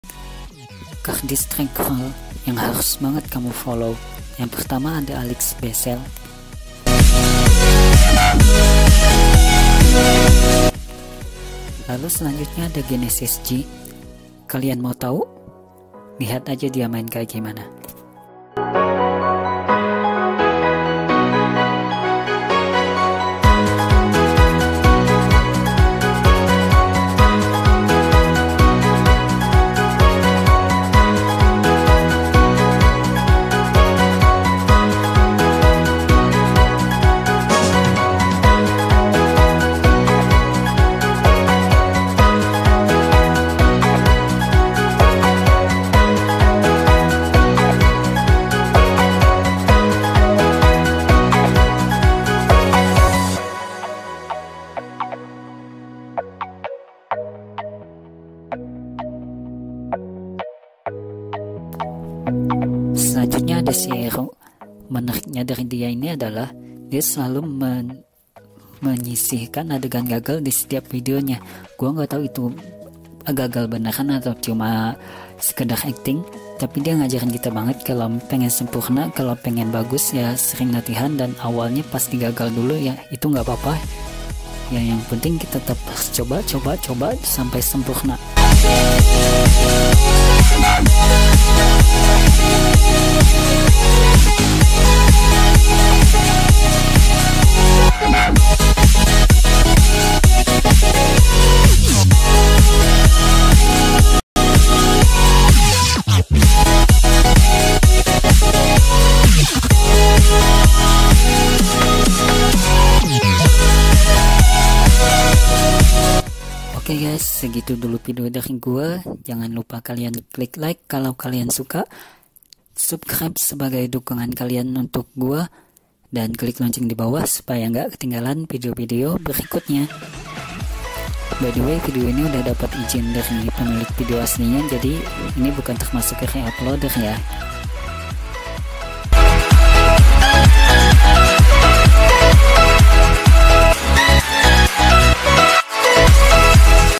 cardistry girl yang harus kamu Follow //instalgram compilation/
Disini bang Isai bakal nunjukkin beberapa cewek cantik dari belahan dunia lain yg jago bgt main nya...  Mngkin saat ini hanya bisa tiga org dulu,..  So pantengin aja yg pastinya kamu bakal terkecak kagum dgn skill  mereka...
 btw guys, video ini udah mendapatkan izin dari pemilik video nya ya...

soo... ini bukan reuploader yh...

keep supporting and keep connect...
lapak online
(ngabisin stok, kartu remi bicycle karnival fatal.